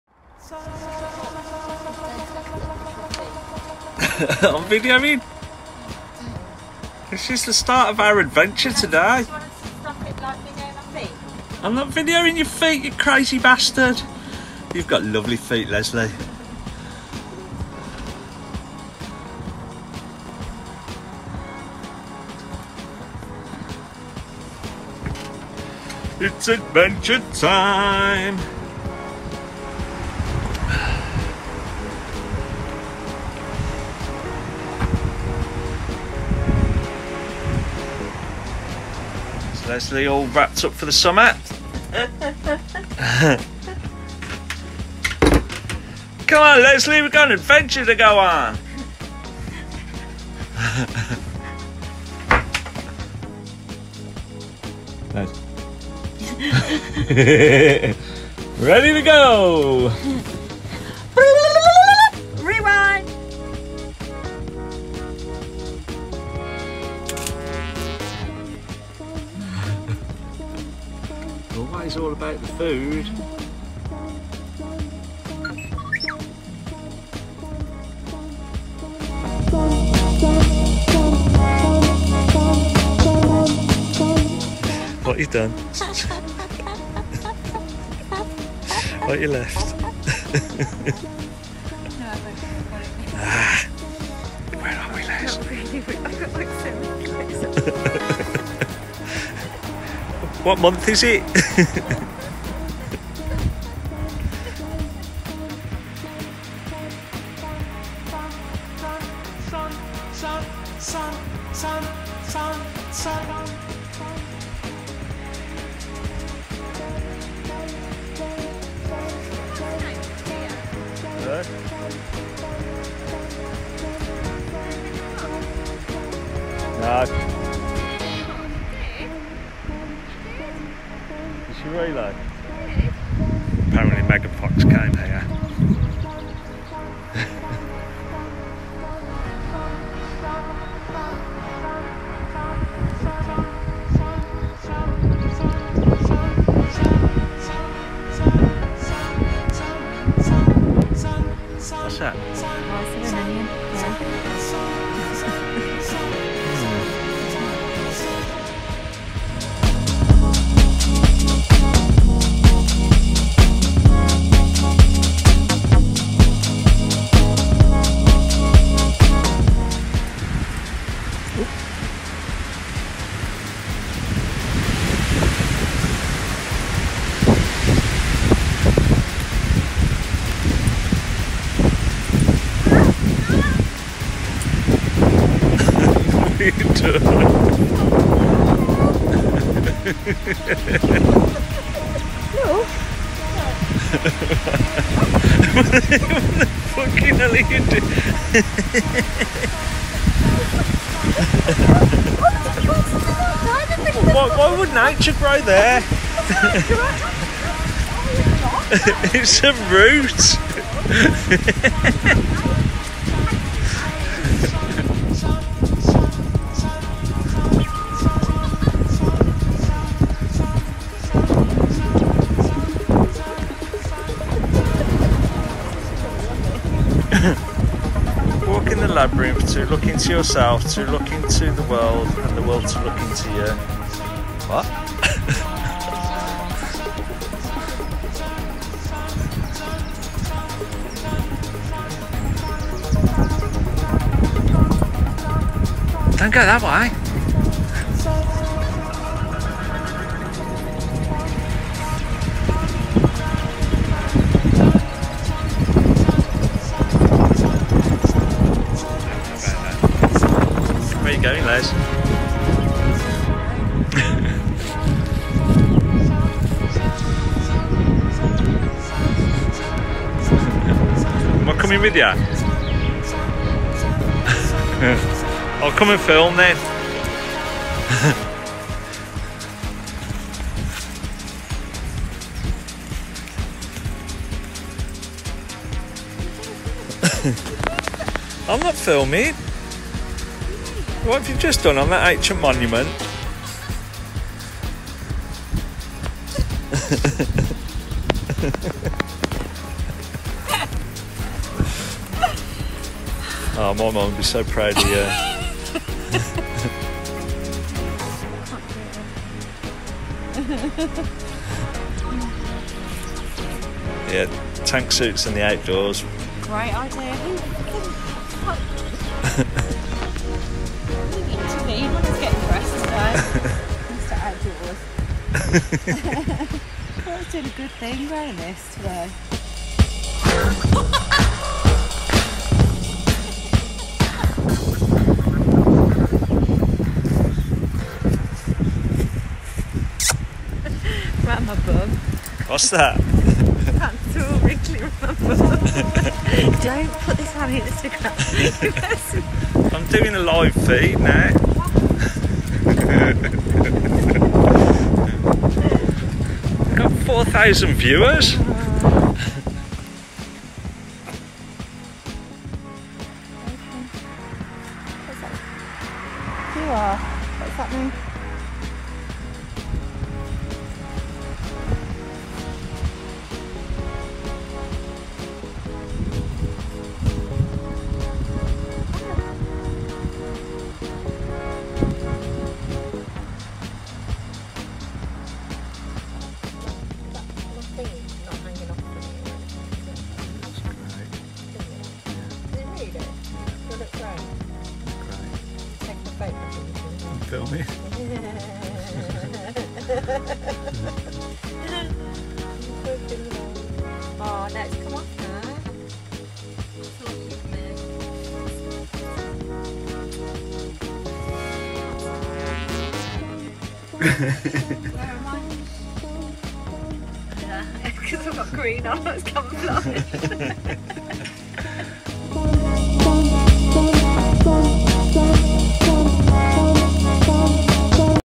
A Trip To The Rollright Stones
We fancied a day out to get away from the Covid19 madness & to enjoy some shroom fuelled ancient magic.